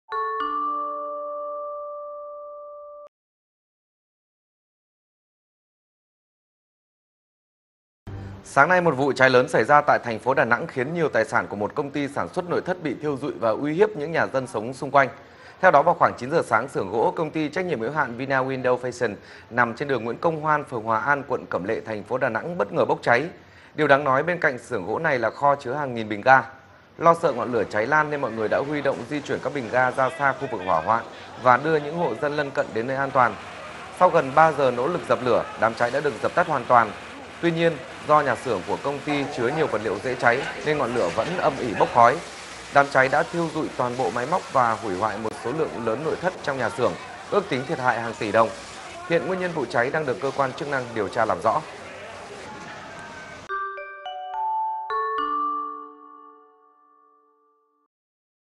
Đà Nẵng: Xưởng gỗ bốc cháy ngùn ngụt, tài sản "bốc hơi"
VTC1 | Xưởng sản xuất gỗ nội thất tại số 273/1, đường Nguyễn Công Hoan, phường Hòa An, quận Cẩm Lệ, Đà Nẵng bất ngờ bốc cháy dữ dội.